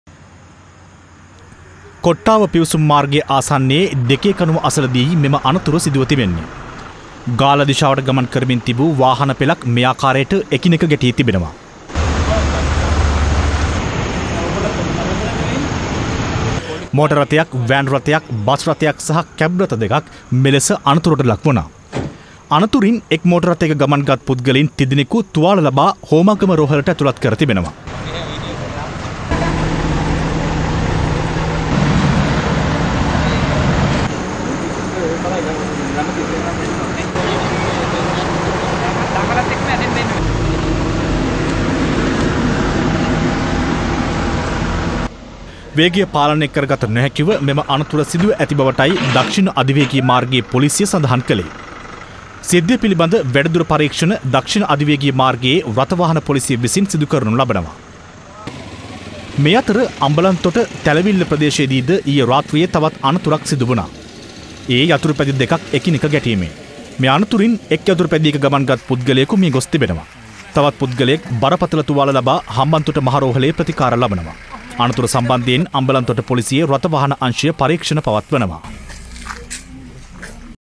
දක්ෂිණ අධිවේගී මාර්ගයේ කොට්ටාව පිවිසුම ආසන්නයේ වාහන 5 ක් එකිනෙක ගැටේ
දක්ෂිණ අධිවේගී මාර්ගයේ කොට්ටාව පිවිසුම ආසන්නයේ ඊයේ රාතී‍්‍රයේ රිය අනතුරක් සිදු වුණා. වාහන 5 ක් එකිනෙක ගැටීමෙන් සිදුවු මෙම අනතුරින් තිදෙනෙක් තුවාල ලැබු අතර ඔවුන් ප‍්‍රතිකාර සඳහා ඇතුලත් කෙරුණේ හෝමාගම රෝහලටයි.

කොට්ටාව පිවිසුම් මාර්ගය ආසන්නයේ දෙකේ කණුව අසලදීයි මෙම අනතුර සිදුව තිබෙන්නේ.ගාල්ල දිශාවට ගමන් කරමින් තිබූ වාහන පෙළක් මේ ආකාරයට එකිනෙක ගැටී තිබෙනවා. මෝටර් රථයක්, වෑන් රථයක්, බස් රථයක් හා කැබ් රථ දෙකක් මෙලෙස අනතුරට ලක් වුණා. අනතුරින් එක් මෝටර් රථයක ගමන් ගත් පුද්ගලයන් තිදෙනෙකු තුවාල ලබා හෝමාගම රෝහලට ඇතුළත් කර තිබෙනවා.

වේගය පාලනය කර ගත නොහැකිව මෙම අනතුර සිදුව ඇති බවටයි දක්ෂිණ අධිවේගී මාර්ගයේ පොලීසිය සඳහන් කලේ. සිද්ධිය පිළිබඳ වැඩිදුර පරීක්ෂණ දක්්ෂිණ අධිවේගී මාර්ගයේ රථ වාහන පොලීසිය විසින් සිදු කරනු ලබනවා. මේ අතර අම්බලන්තොට, තැලවිල්ල ප‍්‍රදේශයේදී ද ඊයේ රාත‍්‍රීයේ තවත් අනතුරක් සිදුවුණා. ඒ යතුරුපැදි දෙකක් එකිනෙක ගැටීමෙන්. මේ අනතුරින් එක් යතුරුපැදියක ගමන්ගත් පුද්ගලයෙකු මියගොස් තිබෙනවා. තවත් පුද්ගලයෙක් බරපතල තුවාල ලබා හම්බන්තොට මහ රෝහලේ ප‍්‍රතිකාර ලබනවා. අනතුර සම්බන්ධයෙන් අම්බලන්තොට පොලීසියේ රථ වාහන අංශය පරීක්ෂණ පවත්වනවා.